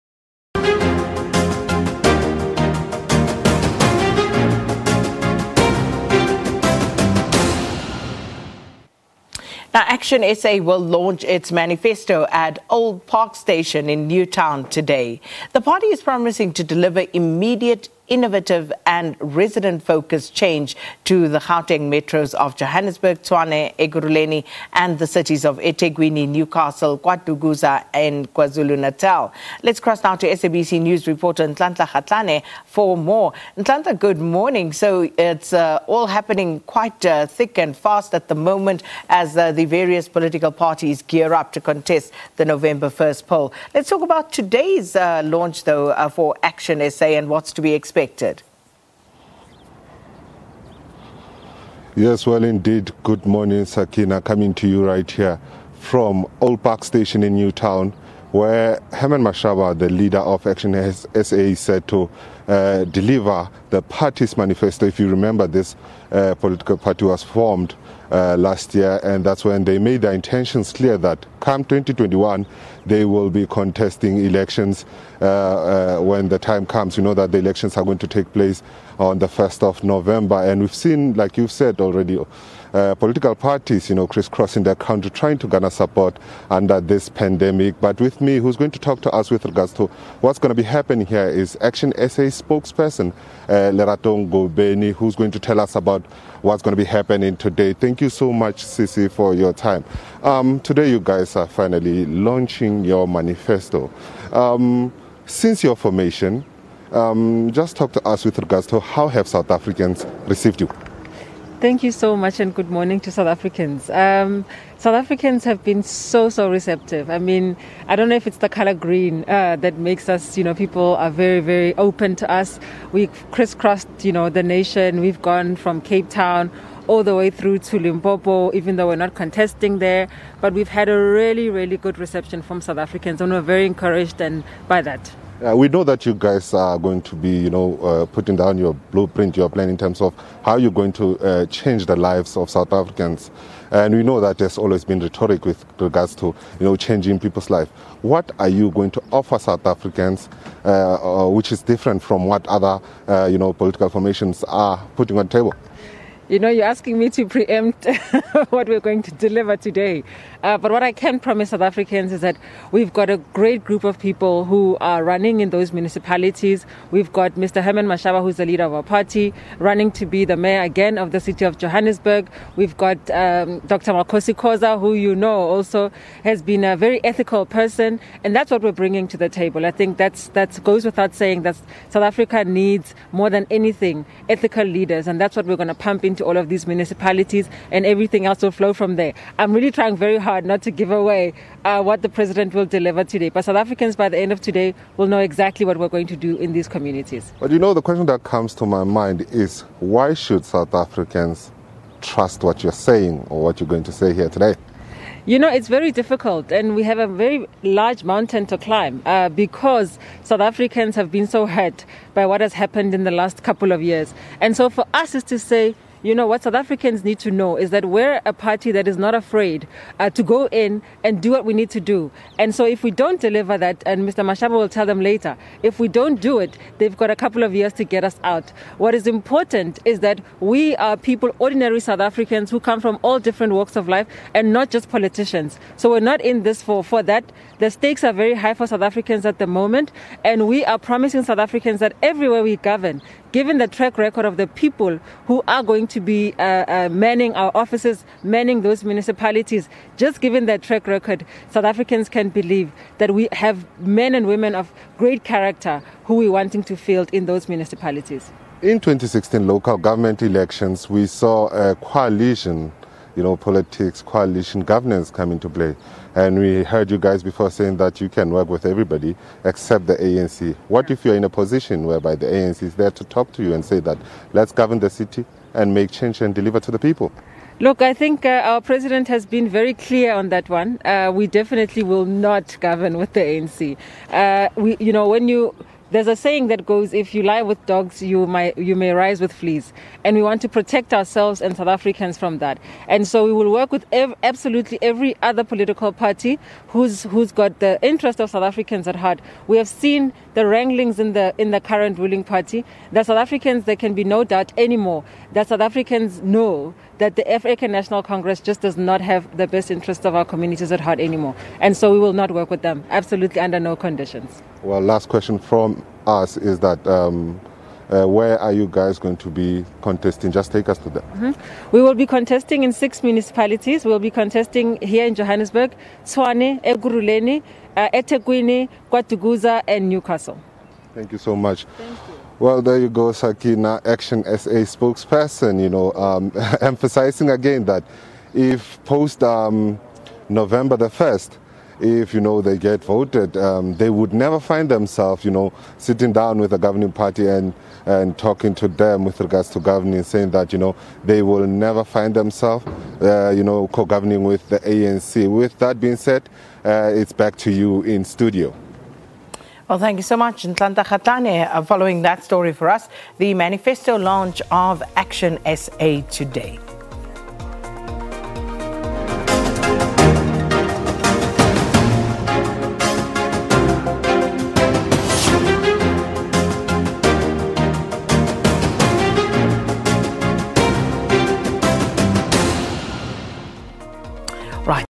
LGE 2021 | ActionSA launches its manifesto in Newtown, Johannesburg
ActionSA will launch its manifesto at Old Park Station, Newtown today. The party is promising to deliver immediate, innovative and resident-focused change to the Gauteng metros of Johannesburg, Tshwane, Ekurhuleni, and the cities of eThekwini, Newcastle and KwaDukuza in KwaZulu-Natal.
Let's cross to SABC NEWS Reporter Ntlantla Kgatlane for more.